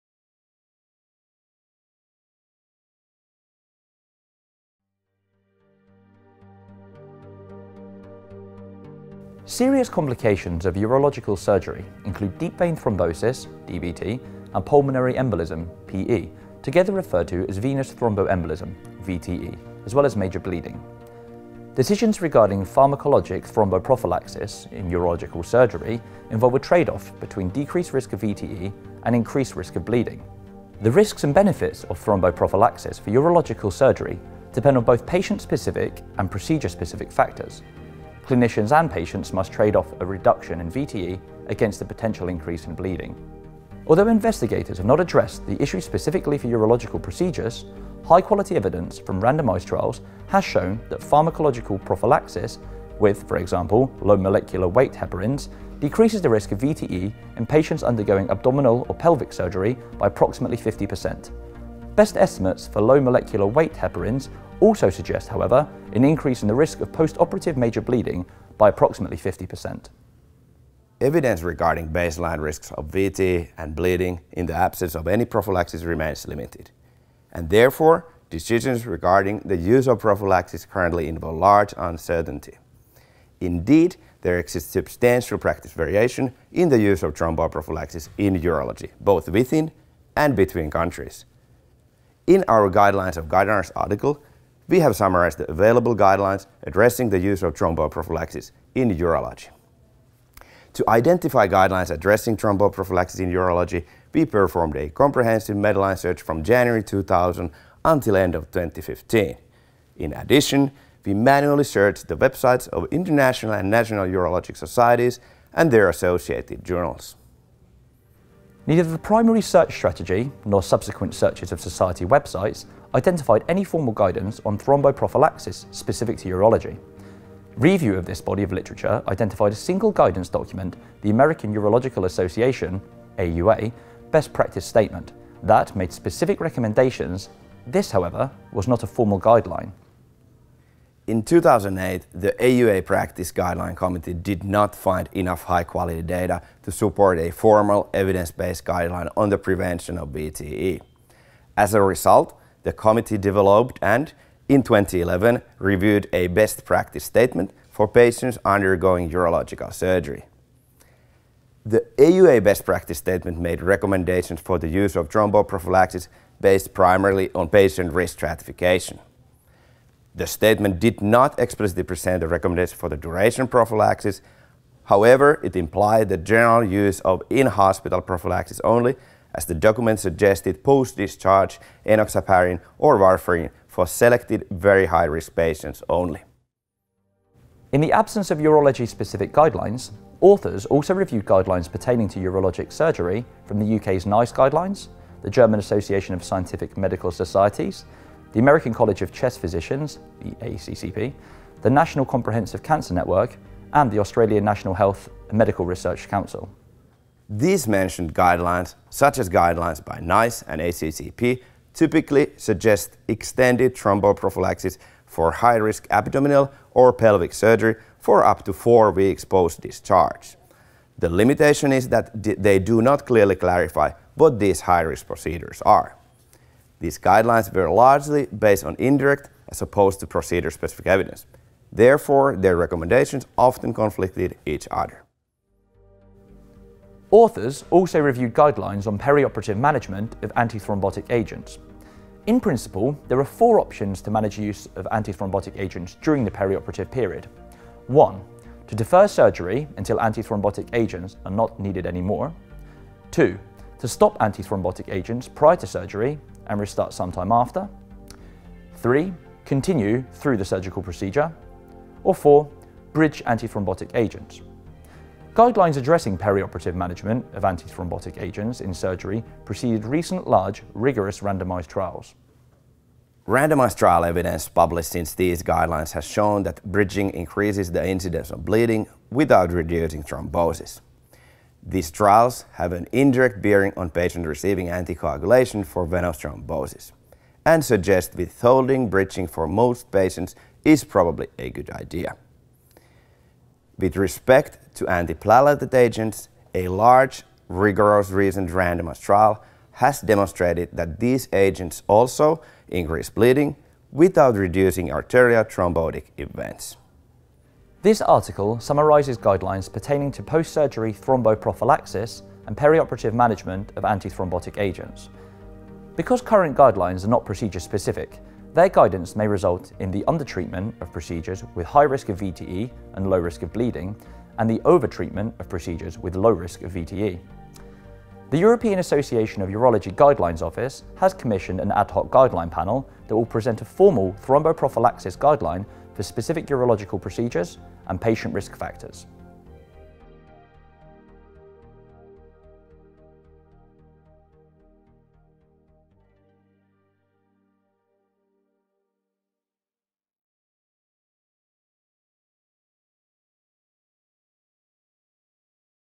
Guideline of guidelines: thromboprophylaxis for urological surgery
Philippe D. Violette, Rufus Cartwright, Matthias Briel, Kari A.O. Tikkinen and Gordon H. Guyatt

BJU International DOI: 10.1111/bju.13496